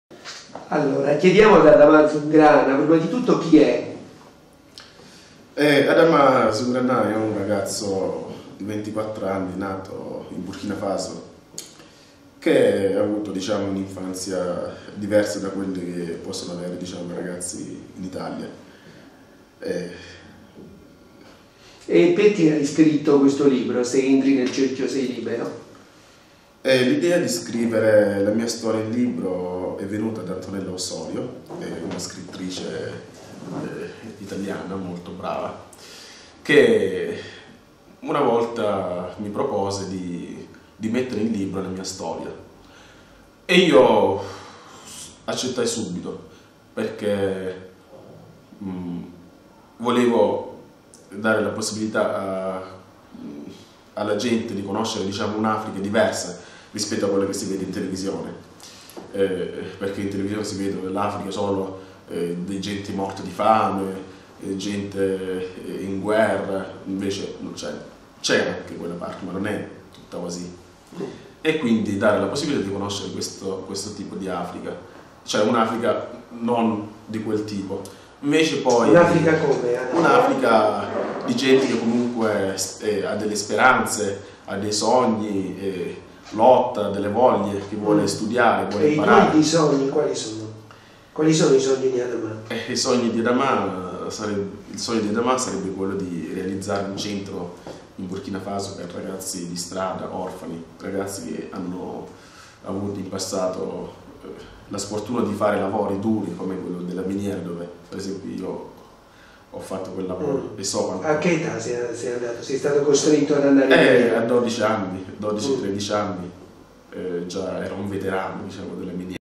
Intervista ad Adama Zoungrana
Intervista ad Adamà Zoungrana, coautore del libro "Se entri nel cerchio sei libero", Rizzoli editore, 2009.